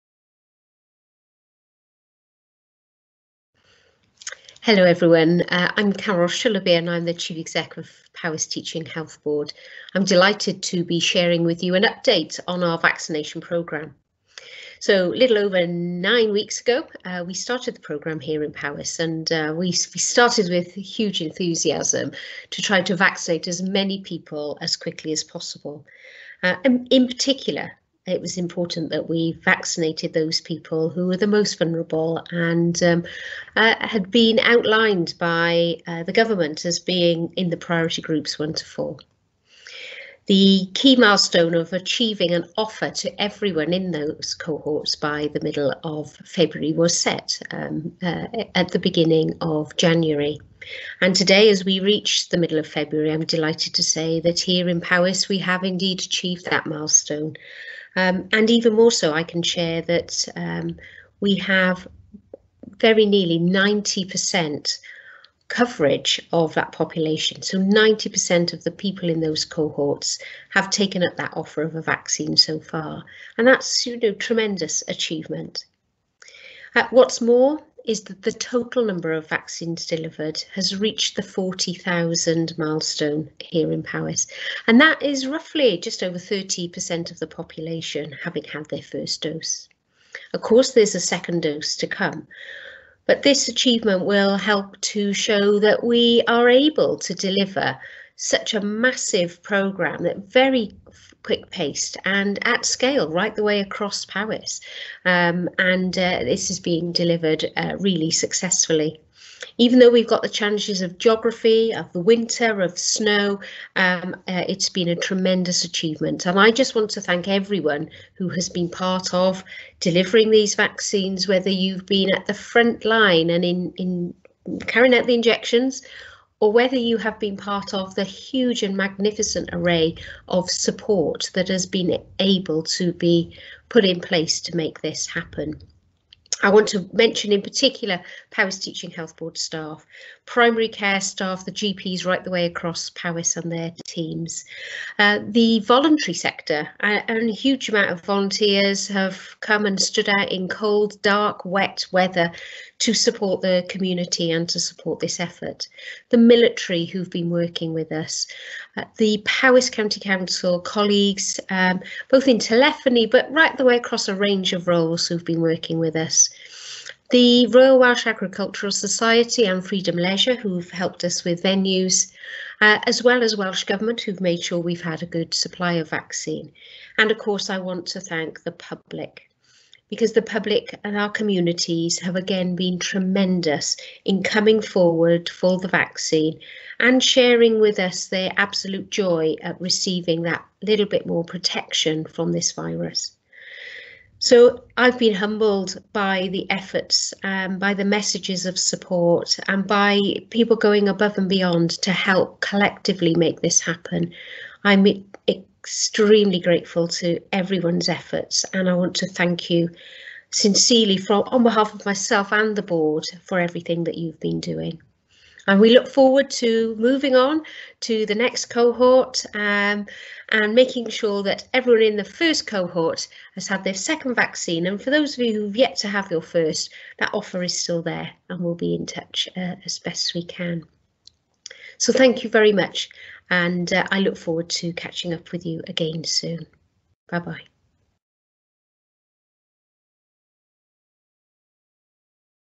Update on COVID 19 vaccination in Powys from Carol Shillabeer
Carol Shillabeer, Chief Executive of Powys Teaching Health Board, provides an update on COVID-19 vaccination in Powys